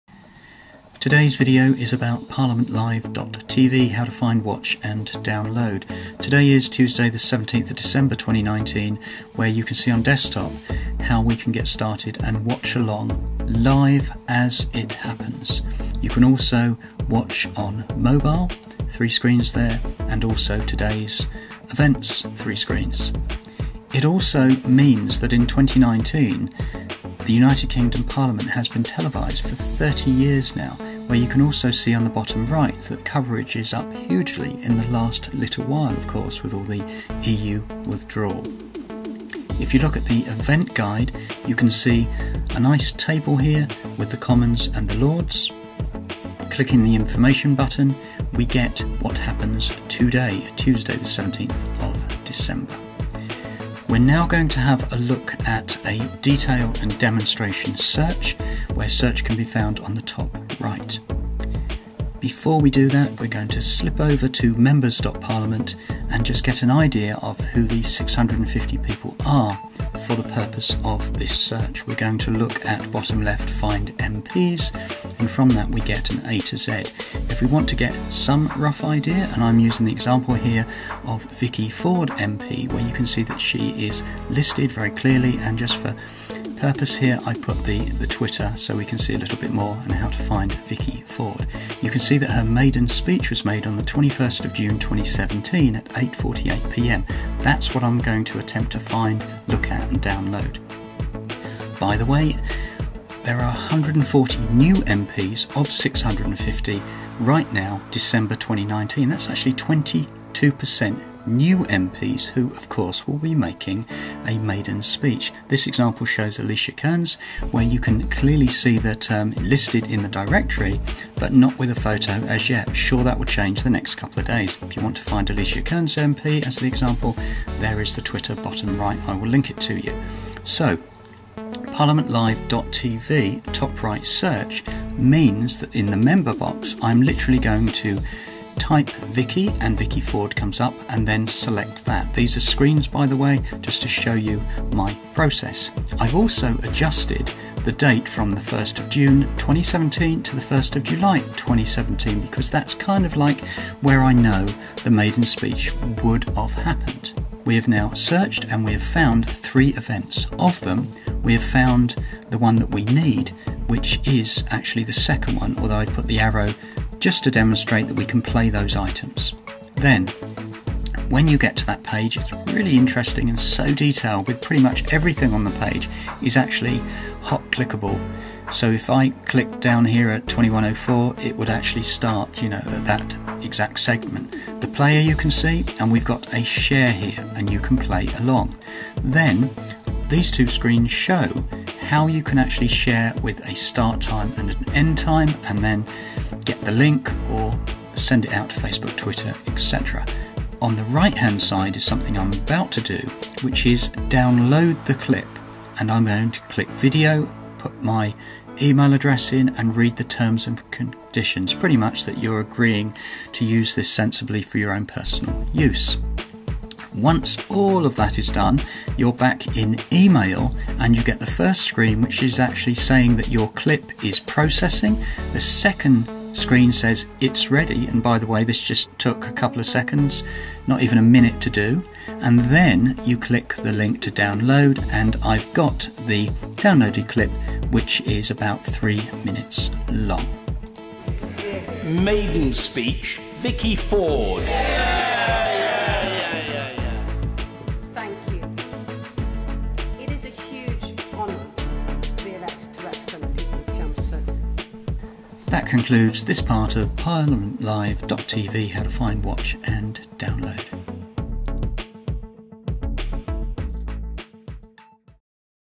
UK Parliament Live TV How to Find, Watch and Download
Today, Tuesday 17 December 2019 is an exciting day for the UK as 650 MPs will return to the House of Commons. Here's a guide on how to find , watch and download.

Vick Ford MP from 1:16
Alicia Kearns MP from 1:40